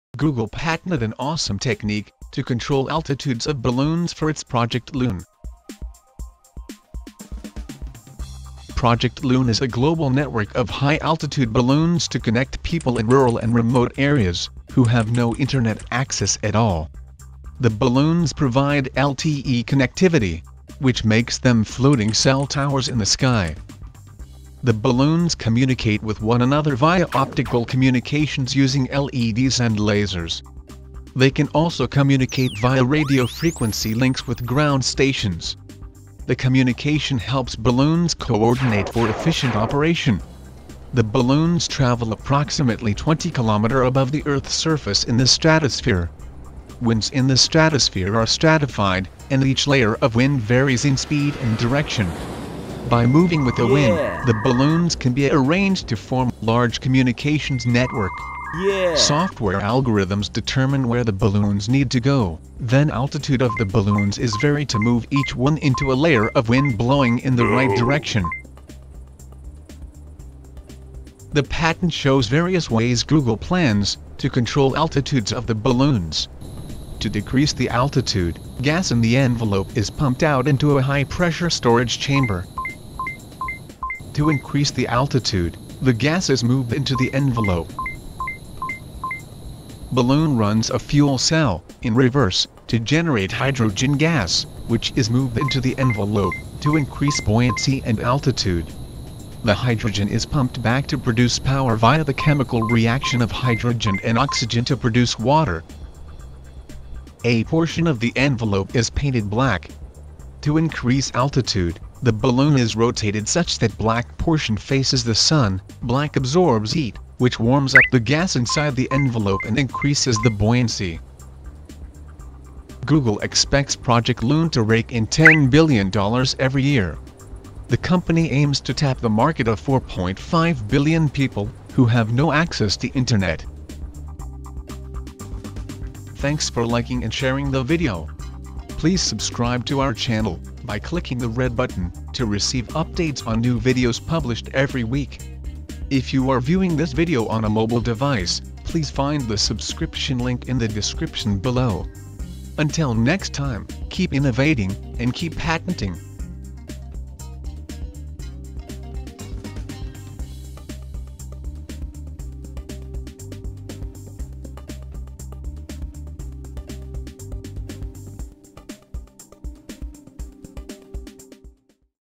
Cómo funcionarán los globos de Google para dar internet en zonas remotas
RevoluTegPlus Tecnológico:
goo.gl/Nk6PwZ - Los globos de Project Loon se viajarán por la estratosfera (a 20 kilómetros del nivel del mar) y proporcionarán conexión a Internet mediante LTE. Además, se comunicarán entre ellos empleando lásers y tecnología LED. Para controlar en qué dirección debe desplazarse cada globo, Google contempla varios métodos en los que hace uso de algoritmos que se encargarán de dirigir el globo hacia la corriente de aire adecuada para desplazarlo en la dirección correcta.